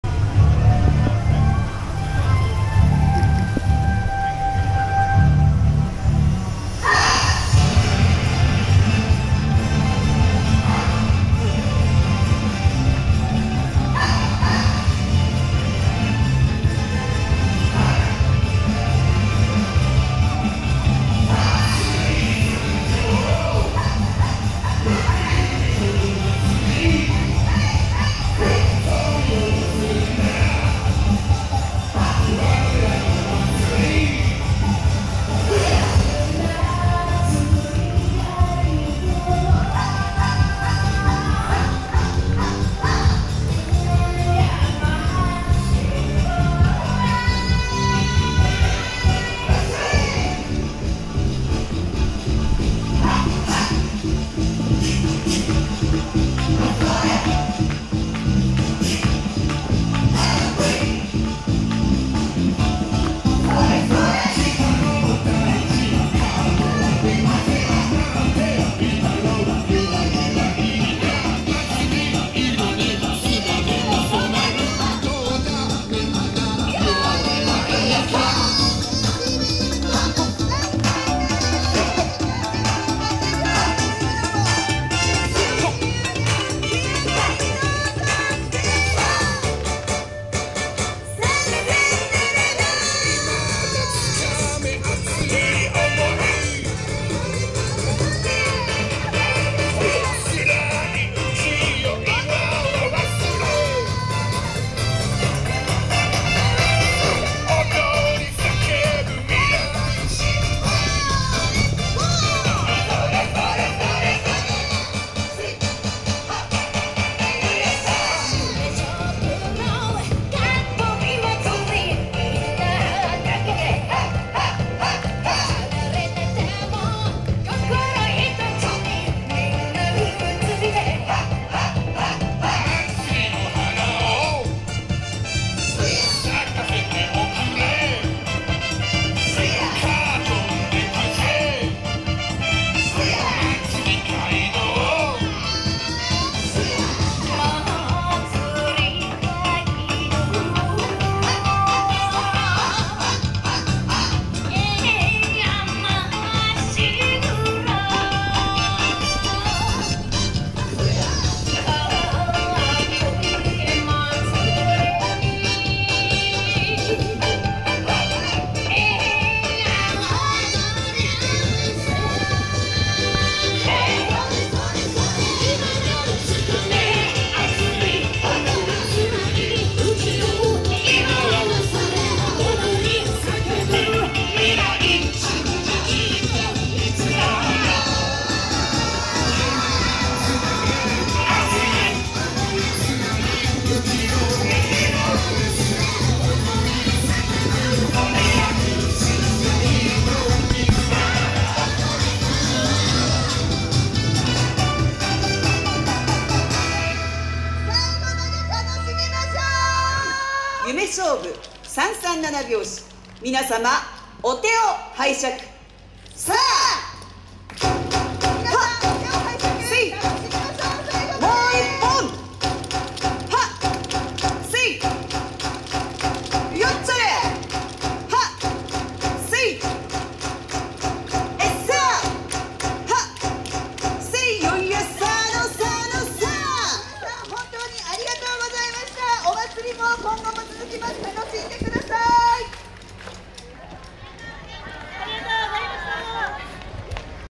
JCB・夢翔舞【4K】／NHK前ストリート／原宿表参道元氣祭スーパーよさこい2022
2022/08/28（令和4年8月28日）
【会場】NHK前ストリート